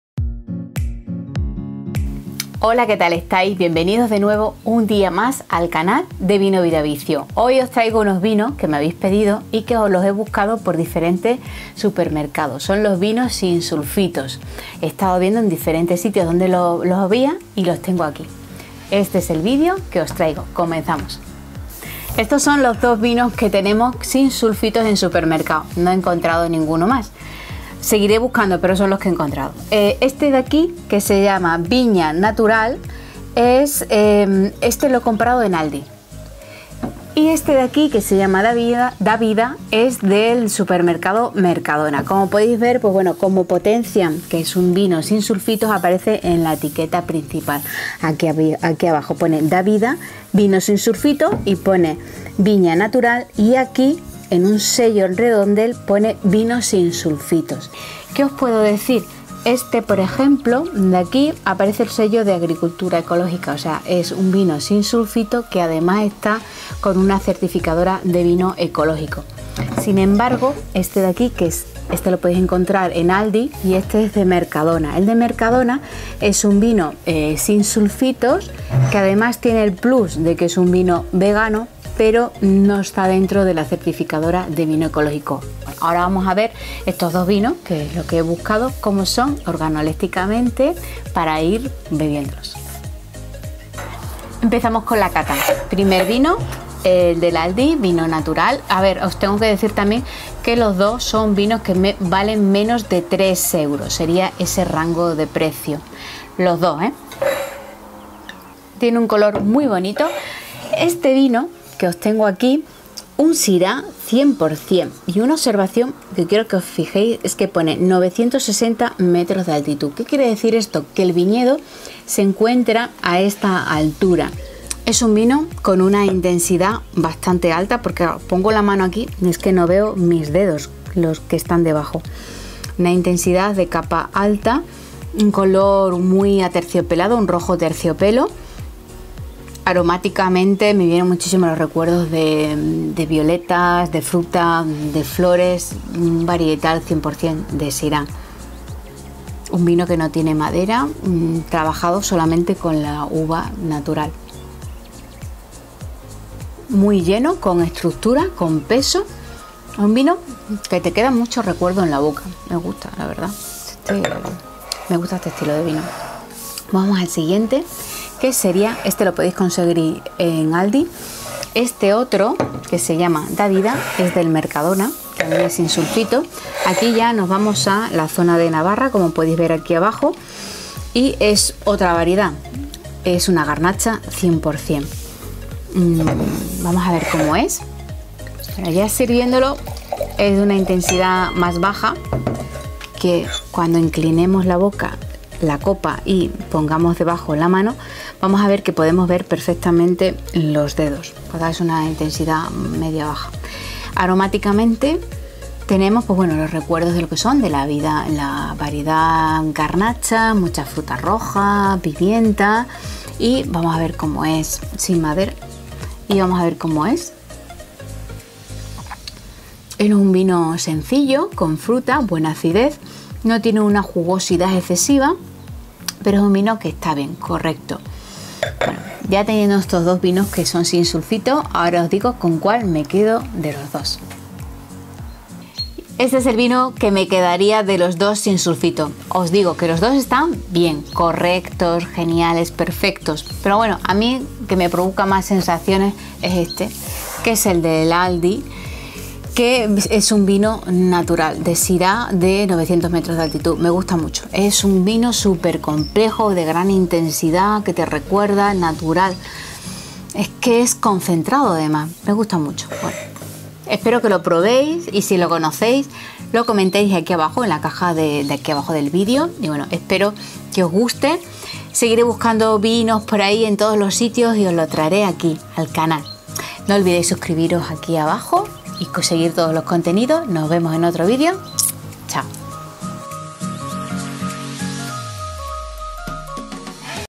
Vino Tinto Sin sulfitos que se pueden comprar en Aldi y Mercadona | Los sulfitos en el vino🍷
Vino Tinto Sin sulfitos que se pueden comprar en Aldi y Mercadona por menos de 3 euros. Cata de estos dos vinos tintos sin sulfitos baratos  que se pueden comprar en el supermercado. Uno de ellos vegano.
Los sulfitos en el vino se pueden evitar. En este vídeo la enóloga Yolanda Hidalgo cata estos dos vinos y nos da su opinión y nos dice con cuál se queda de los dos vinos sin sulfitos
Variedad garnacha.